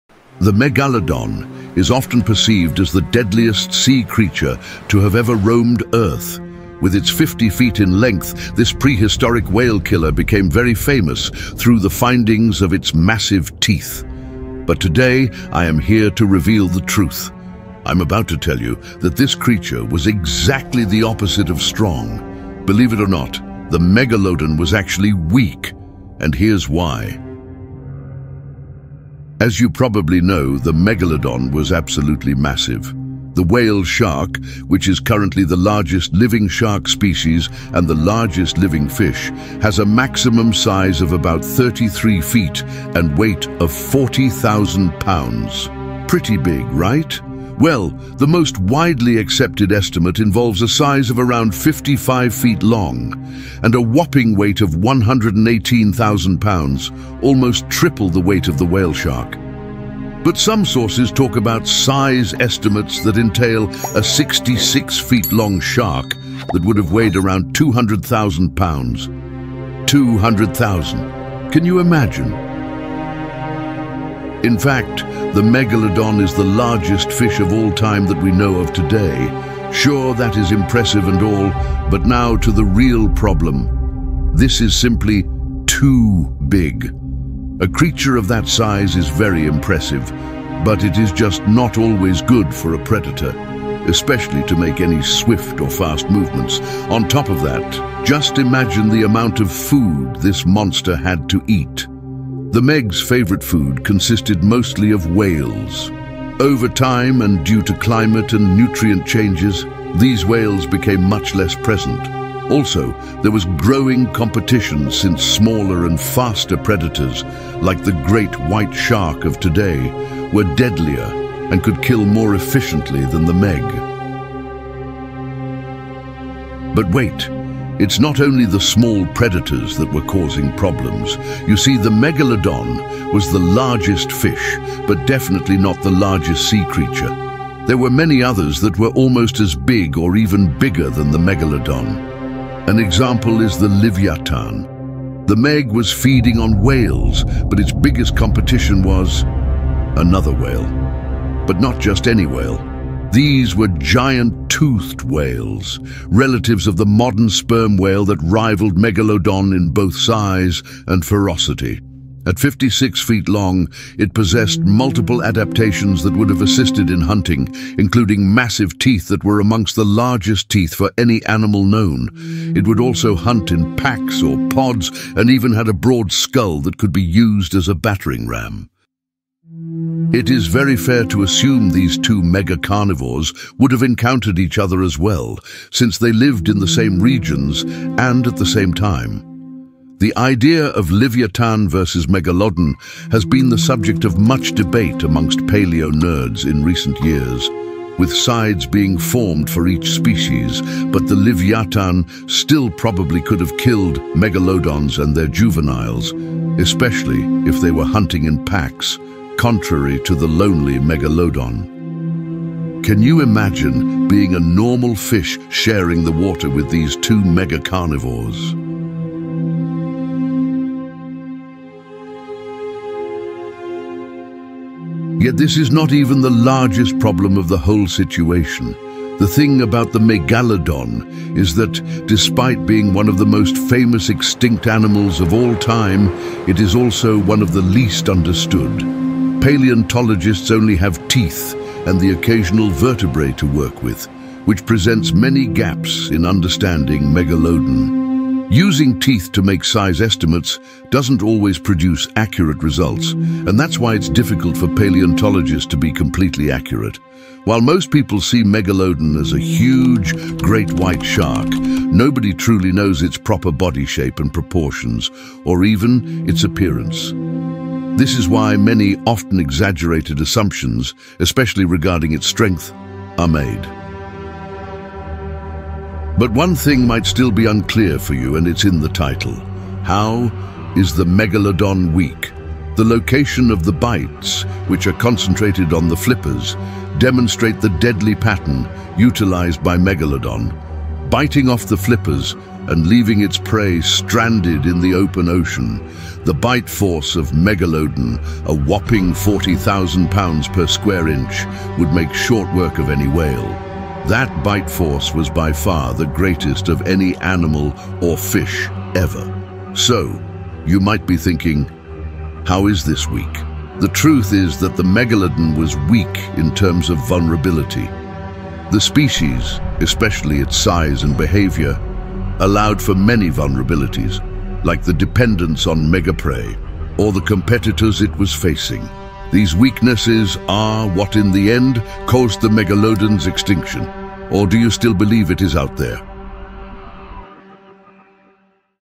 Why the Megalodon was Actually WEAK
The Truth about the Megalodon: The Megalodon was WEAK. The megalodon is often perceived as the deadliest sea creature to have ever roamed Earth. With its fifty feet in length, this prehistoric whale killer became very famous through the findings of its massive teeth. But today I am here to reveal the truth. I’m about to tell you that this creature was exactly the opposite of strong. Believe it or not, the megalodon was actually weak. And here’s why…

Thumbnail: Edited with Parts of a Picture by Qbliviens

SOURCES
(main source): this source was used a lot / inspired a lot of the script made for this video

These videos, especially the first two, were videos used in this YouTube video. You can check them out under these links.

PICTURES
Thumbnail (Megalodon Render): Mamlukzilla on DeviantArt
I do not take credit for any of these pictures.